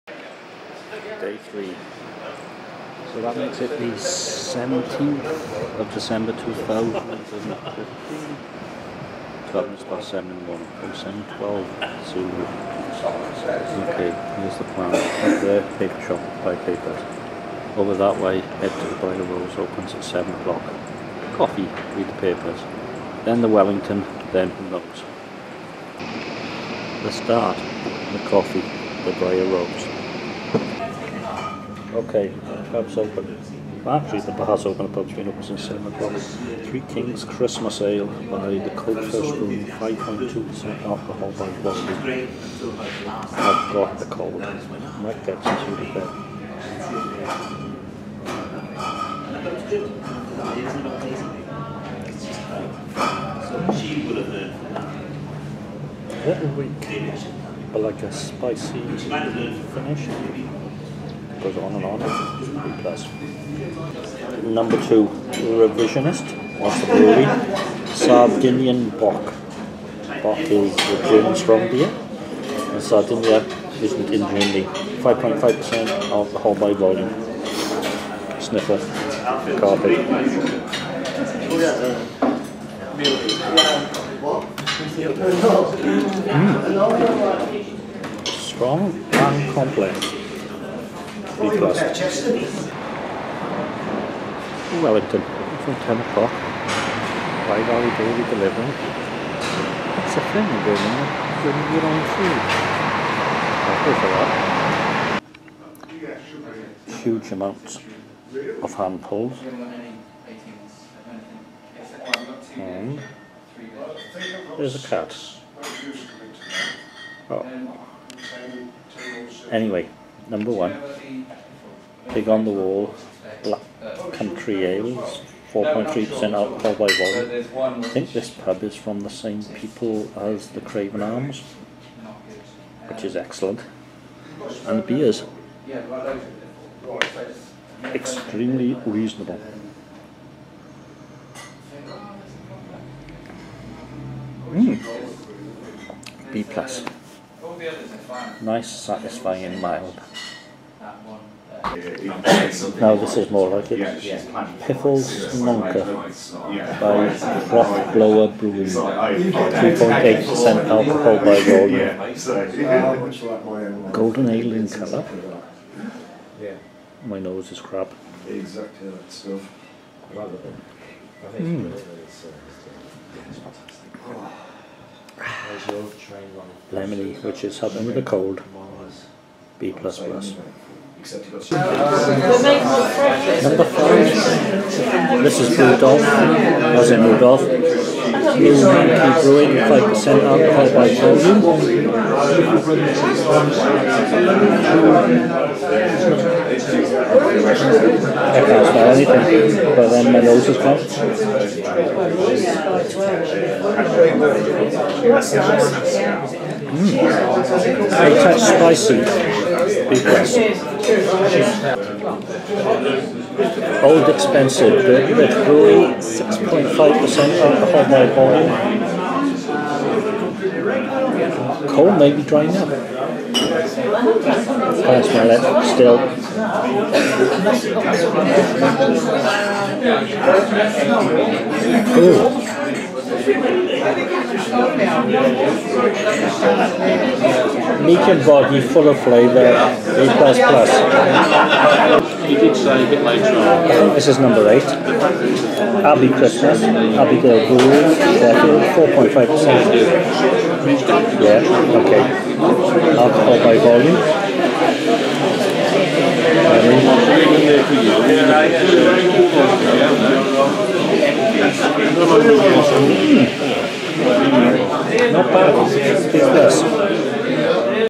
Day 03 - Sniffle and Snonker
A day of halves, infact all my beers were half pints. The Briar Rose and the rather wonderful Wellington. Birmingham, I will be back but minus my cold.